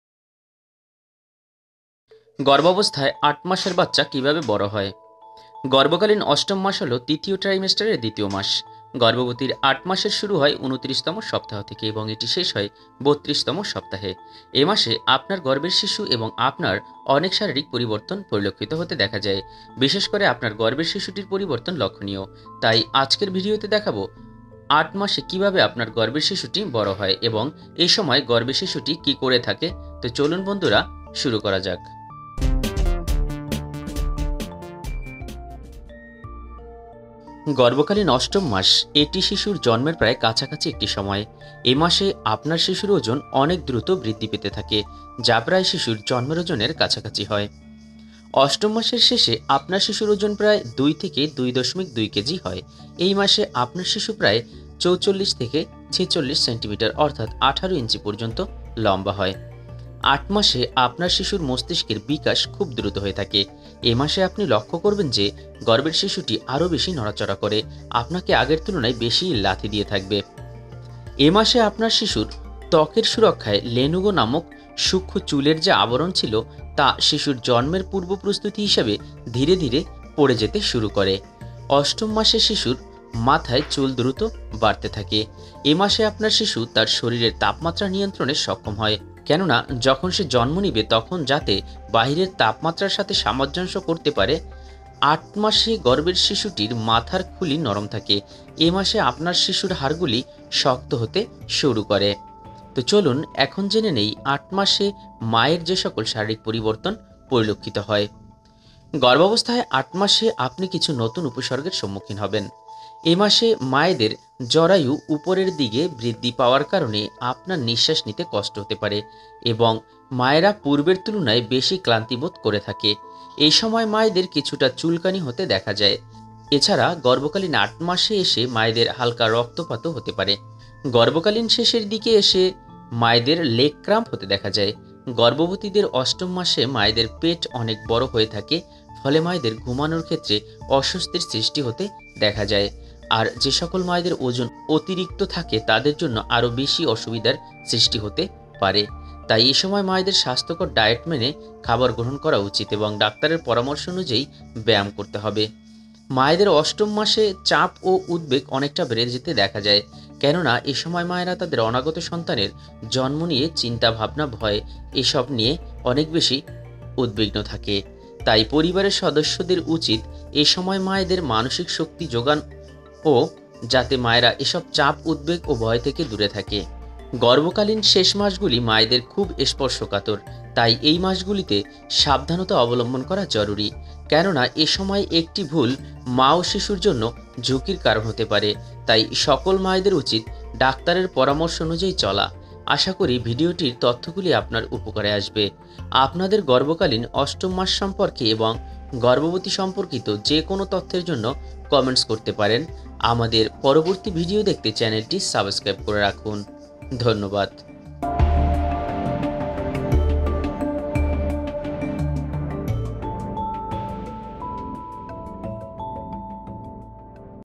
গর্ভাবস্থায় ৮ মাসের বাচ্চা কীভাবে বড় হয় এবংমায়ের শারীরিক পরিবর্তন||8 Month Baby Development.
গর্ভকালীন অষ্টম মাস হলো তৃতীয় ট্রাইমিস্টারের ২য় মাস। গর্ভবতীর ৮ মাসের শুরু হয় ২৯তম সপ্তাহ থেকে এবং এটি শেষ হয় ৩২ তম সপ্তাহে। এ মাসে আপনার গর্ভের শিশুর এবং আপনার অনেক শারীরিক পরিবর্তন পরিলক্ষিত হতে দেখা যায়। বিশেষ করে আপনার গর্ভের শিশুটির পরিবর্তন লক্ষণীয়। তাই আজকের ভিডিওতে দেখাবো যে ৮ মাসে কিভাবে আপনার গর্ভের শিশুটি বড়ো হয় এবং এ সময় গর্ভে শিশু কি করে থাকে তো চলুন দেখে নেয়া যাক

ভিডিওটি  যদি আপনাদের ভালো লেগে  থাকে অবশ্যই লাইক দিবেন কমেন্টস করবেন আর শেয়ার করতে ভুলবেন না।

Photo: Google Photo